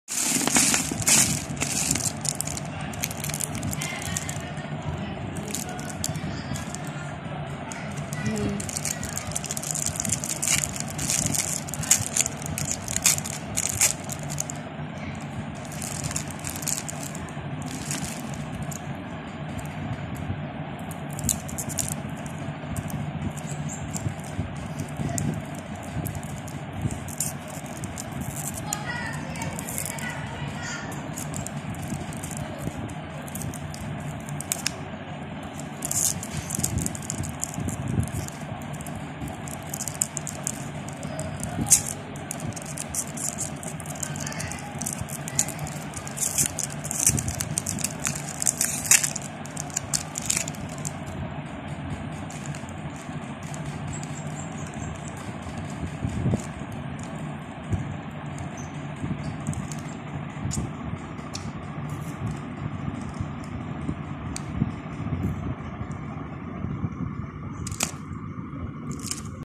Amazing Lollipop opening ASMR || Yummy Kandy
Hi, I really love lollipops and chocolate eggs. I unpack Kinder surprises and ASMR candies every day.